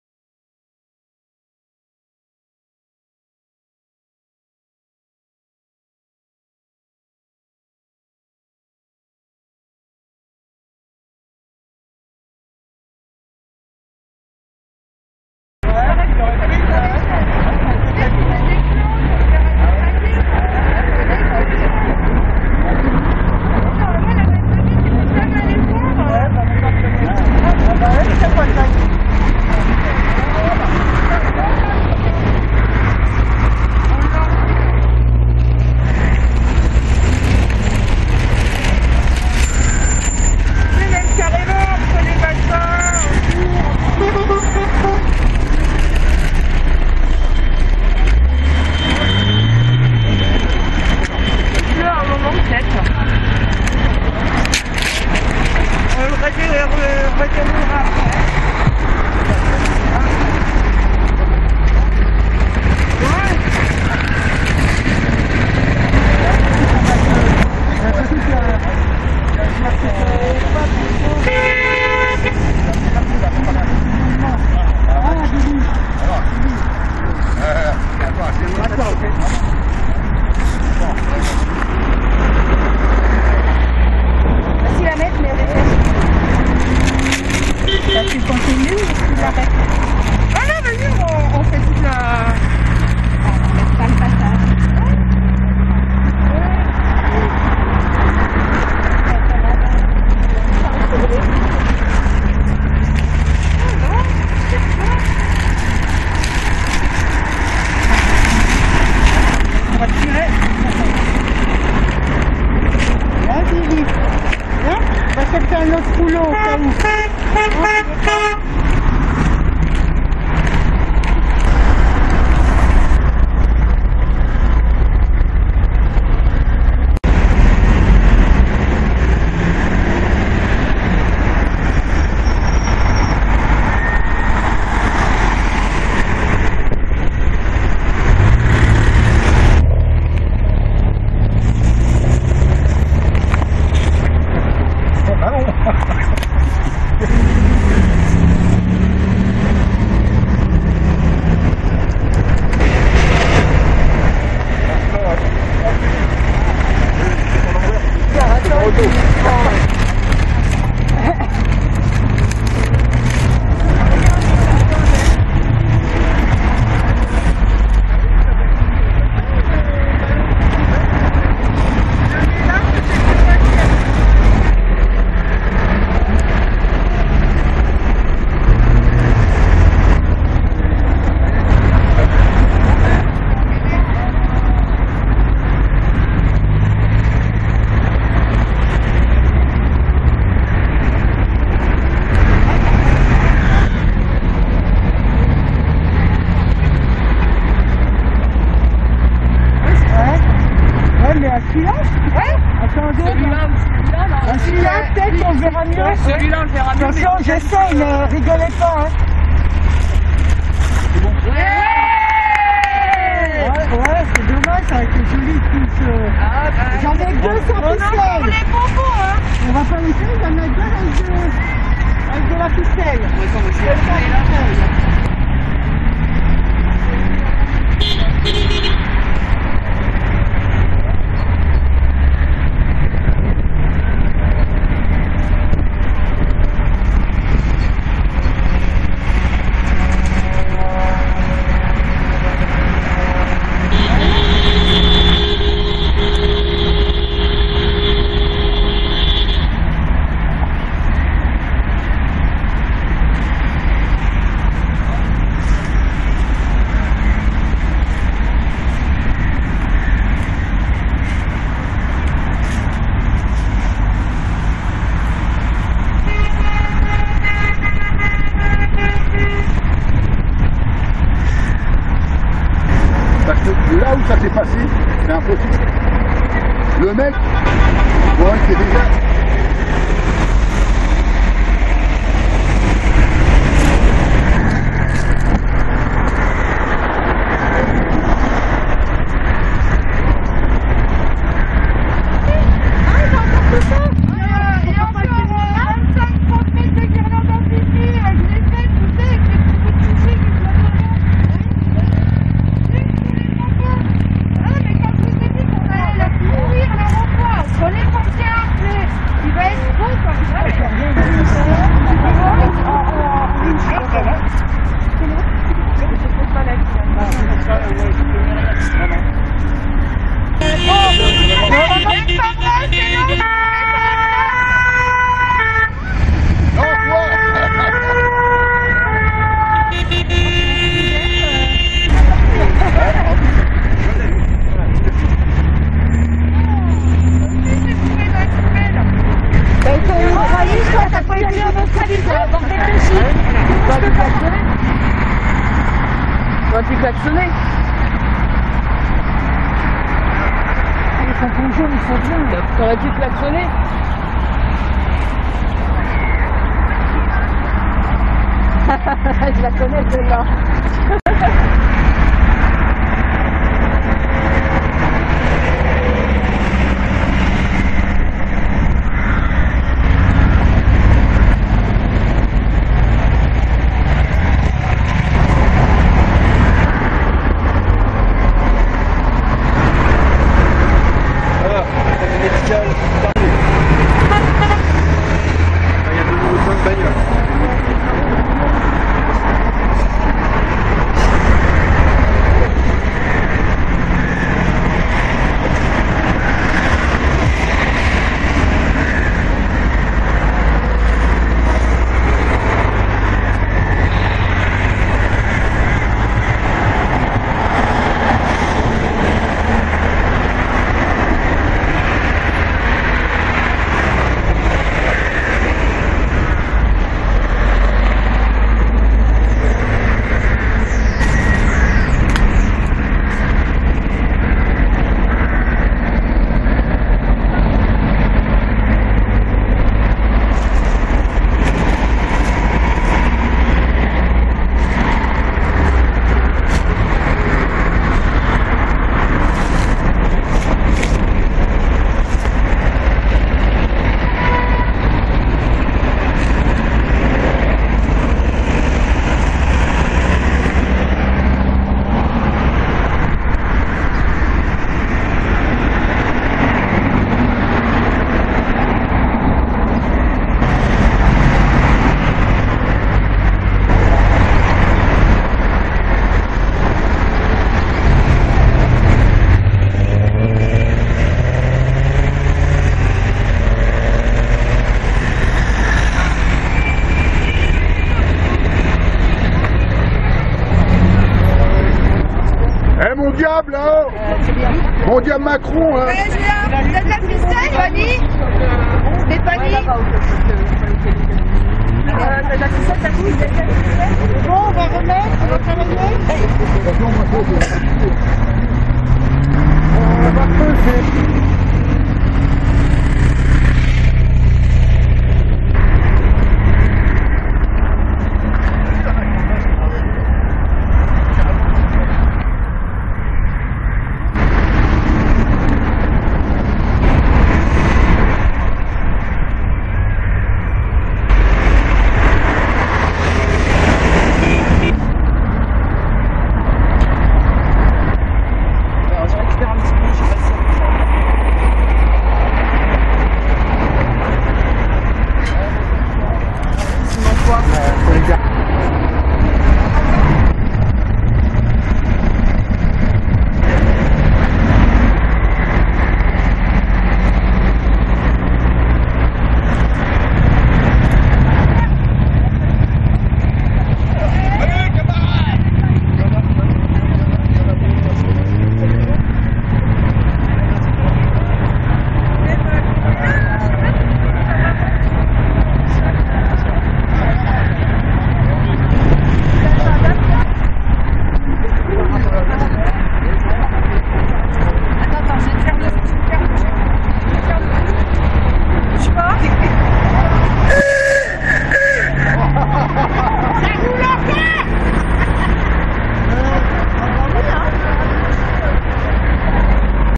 Gilets jaunes - Auxerre 89 Yonne - Acte #79, fin de confinement.
Retour au rond-point pour décoration signalétique en jaune :-)